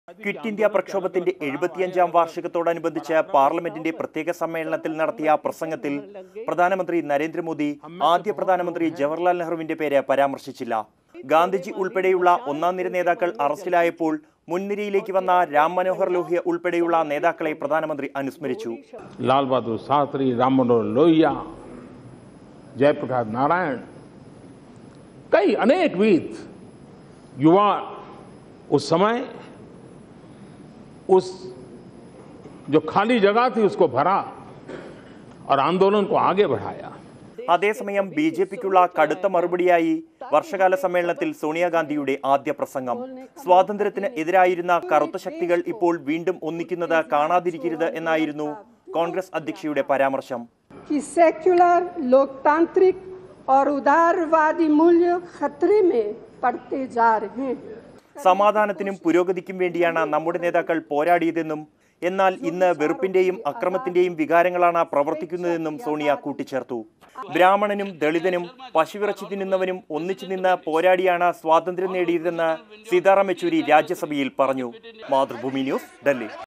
Modi Omits Nehru, Congress In Quit India Speech| Mathrubhumi News
New Delhi: Terming poverty, lack of education and malnutrition as the greatest challenges, Prime Minister Narendra Modi today called for special initiatives to end all such ills in the next five years with the pledge of 'Karenge Aur Kar Ke Rahenge' (We will do and surely do). In his speech, PM Modi omitted references to first prime minister Jawaharlal Nehru and Congress and spoke of Gandhiji and Ram Manohar Lohia. An irked Congress chairperson Sonia Gandhi took a veiled dig at the BJP and RSS by saying that those dark forces which were against the freedom struggle have have come together to destroy the roots of democracy as "clouds of hate and division" were hovering over secular and egalitarian values.